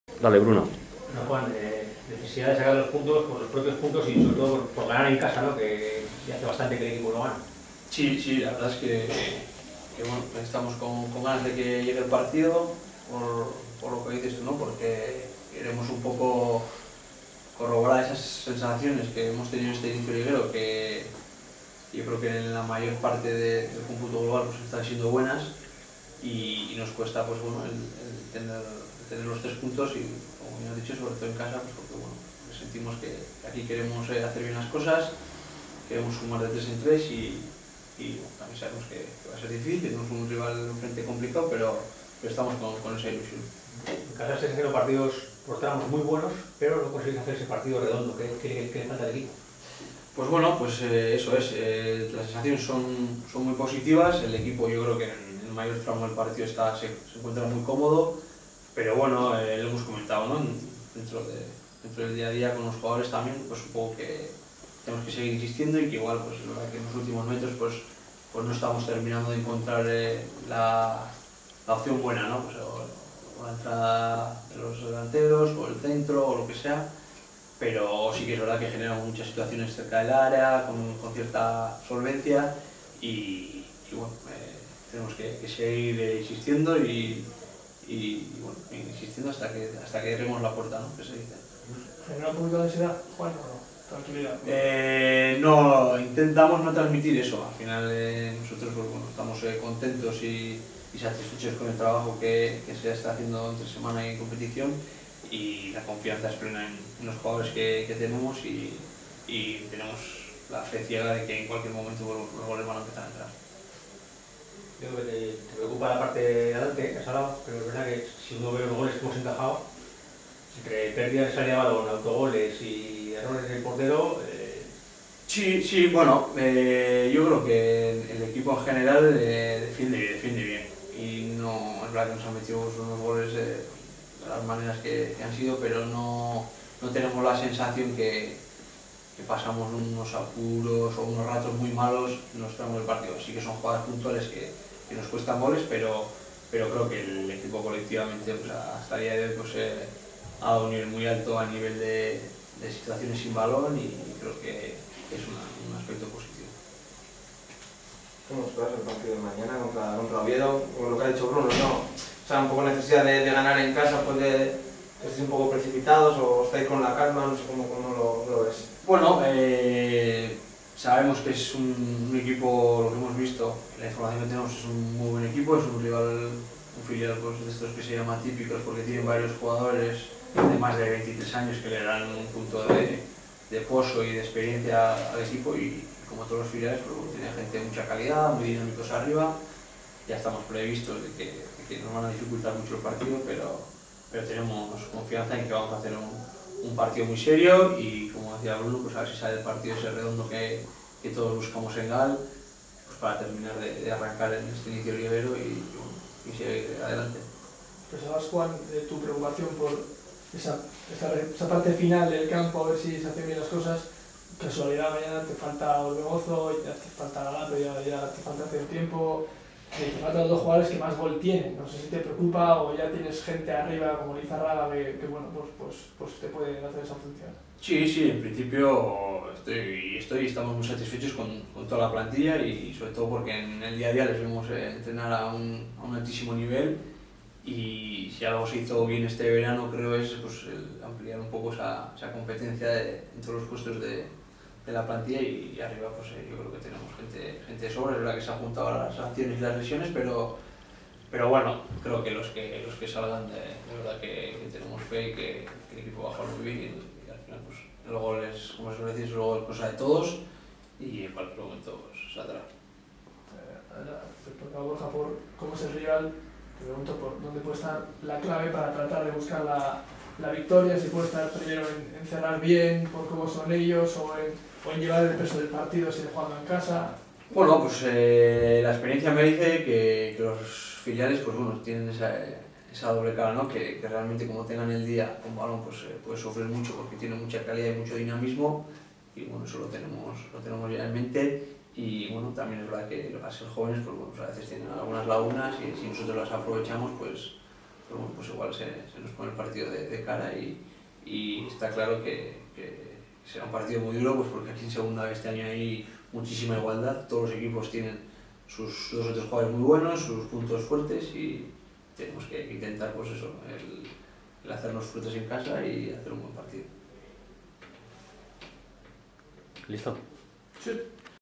Rueda de prensa: Juan Domínguez
Declaraciones previas al partido de liga en Irún frente al Real Oviedo B